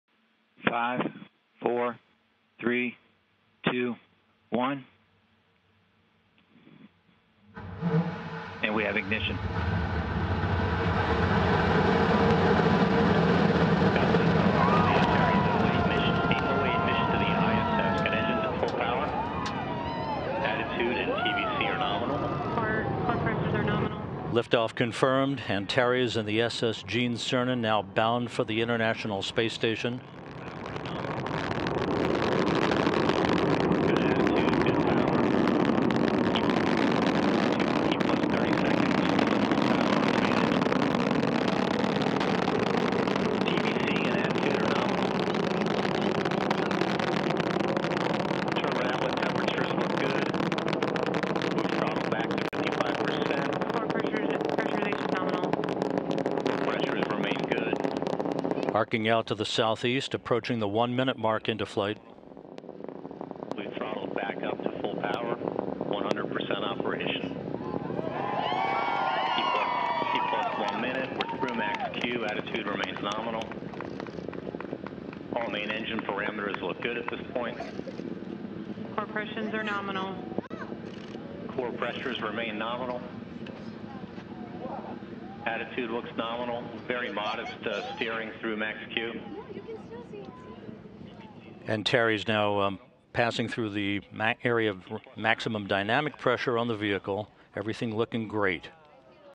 U.S. Commercial Cargo Spaceship Heads to the International Space Station
Orbital ATK’s Antares rocket blasted off Nov. 12 from the Wallops Flight Facility in Virginia to send the company’s Cygnus spacecraft to orbit on the start of a two-day journey to deliver 3 ½ tons of supplies and science experiments to the International Space Station. Dubbed the “SS Gene Cernan” after the late Gemini and Apollo astronaut who was the last man to walk on the moon, Cygnus is scheduled to arrive at the station Nov. 14 where it will be attached to the Unity module for a three-week stay.

This video is available for download from NASA's Image and Video Library